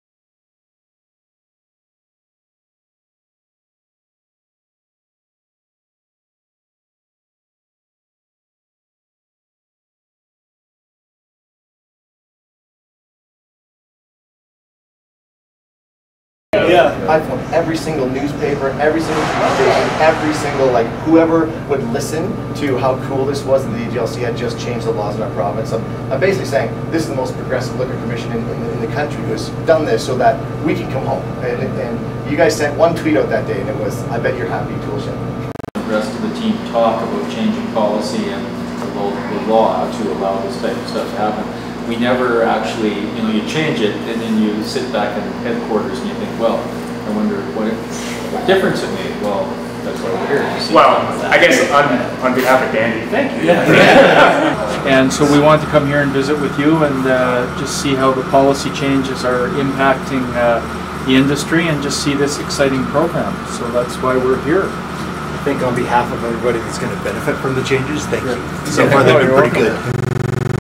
Calgary - Olds brewery tour
Throughout 2015, Alberta’s liquor manufacturing sector was bubbling with activity.

This got us wondering how the AGLC’s policies are working for the industry.

So our Board Chair, Susan Green, President and CEO, Bill Robinson, and Vice President of Liquor Services, Alain Maisonneuve, set out on a late summer ‘beer’ road trip.

Here are the highlights of what they found out.